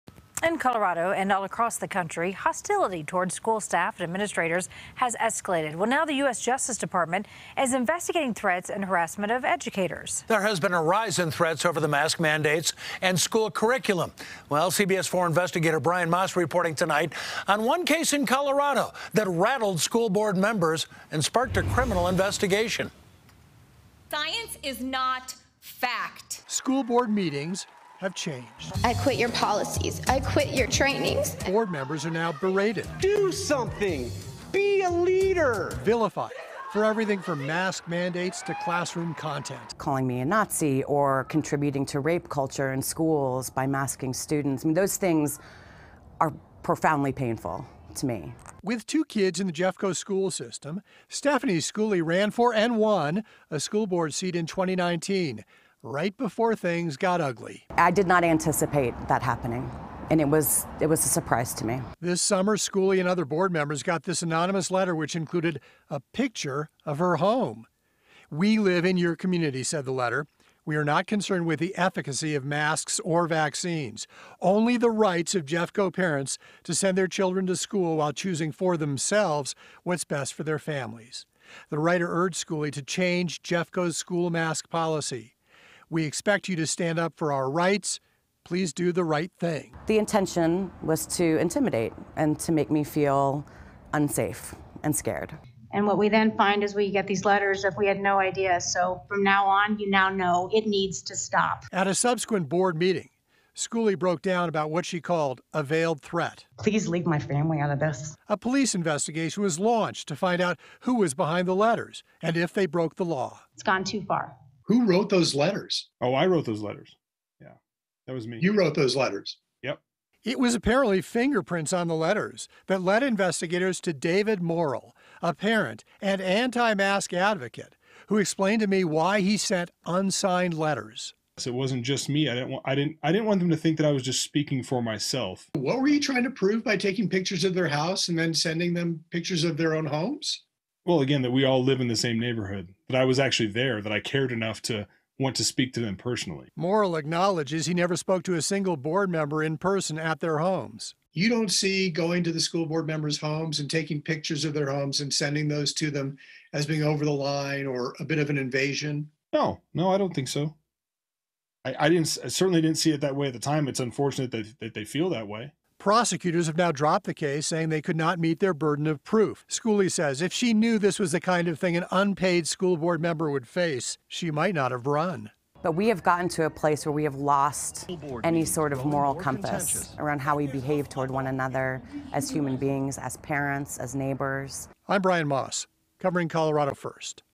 Colorado Man Who Sent Anonymous Letters, Pictures To School Board Members Faces No Charges
The Jefferson County District Attorney does not plan to file criminal charges against an Arvada man who wrote anonymous letters to Jefferson County school board members.